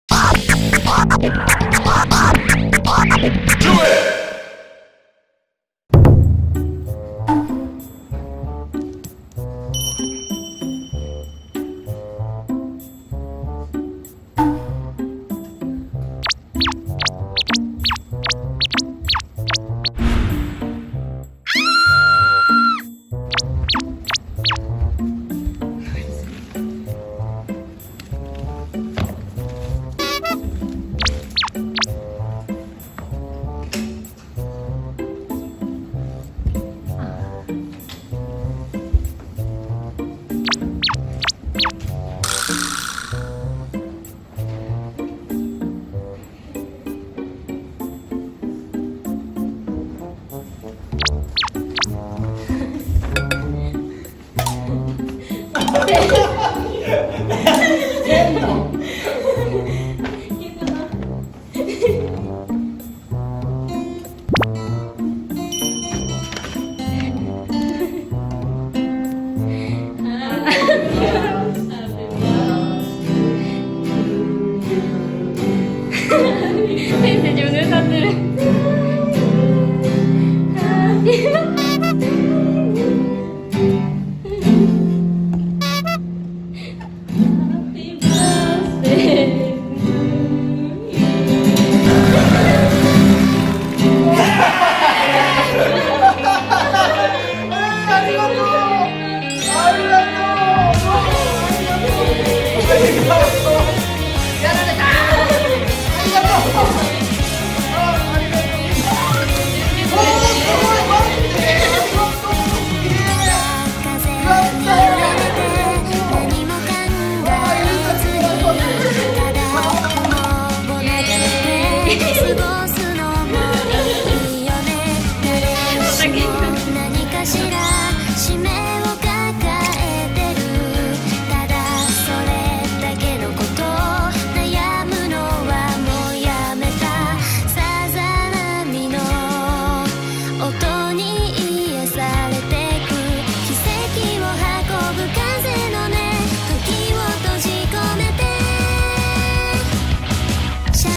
【感涙】弟子が２５万円のギターでお祝いしてくれた!! 誕生日ドッキリ
ガンプラ大好きダンサー

【ガンNAOちゃんねる】

動画内一部の音源はDOVA-SYNDROME様よりお借りしております